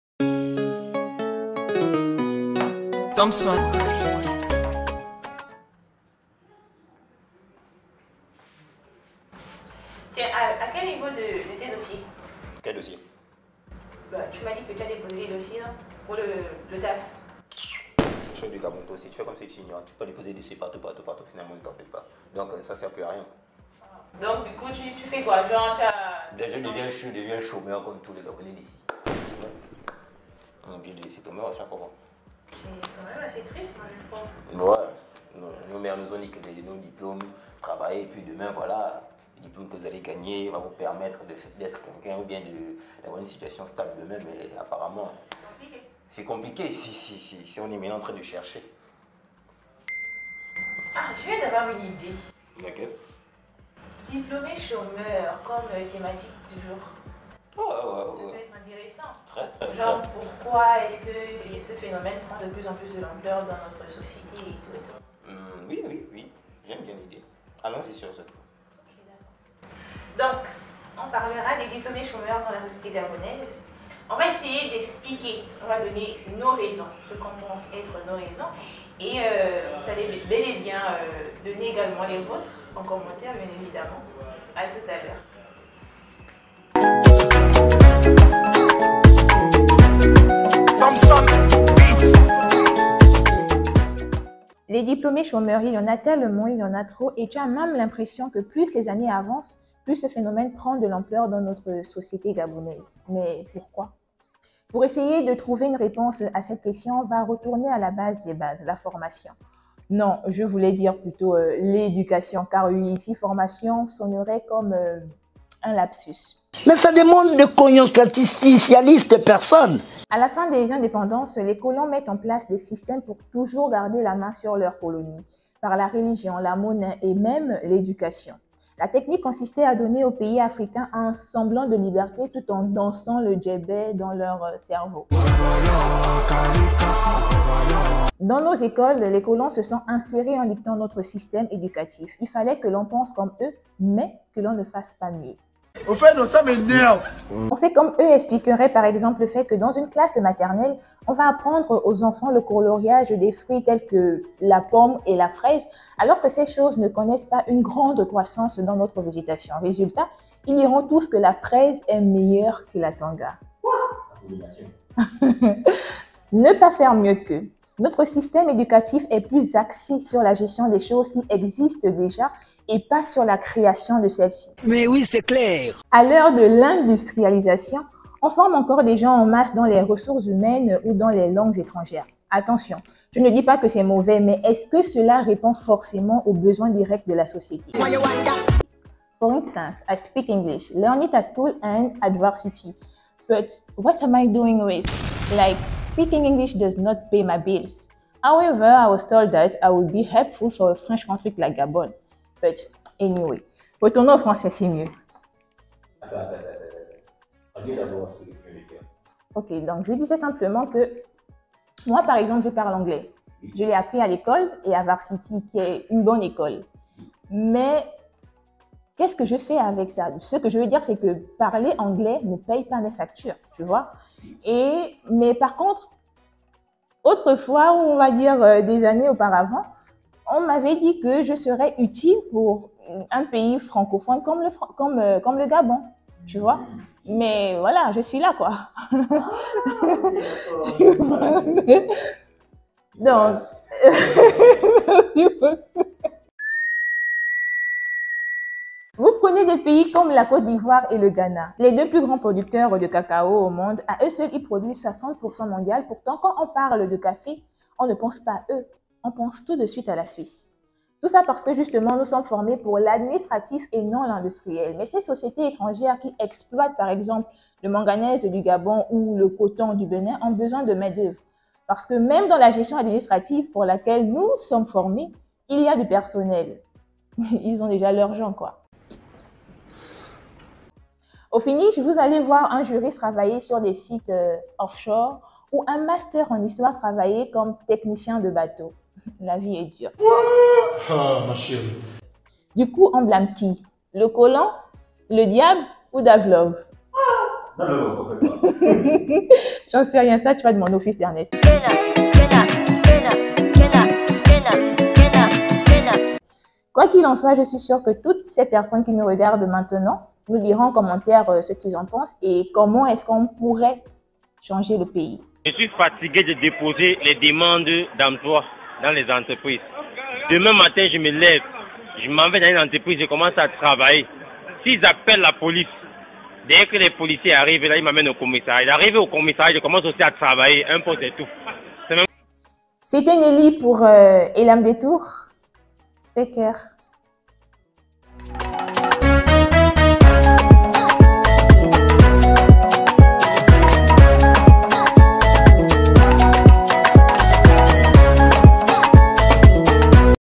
Gabon 🇬🇦: Pourquoi tant de diplomés Chômeurs ? | ElamDetour 6
ElamDetour s'intéresse aujourd'hui au problème des diplomés chômeurs.
Nelly en 5 minutes vous livre son analyse sur la question.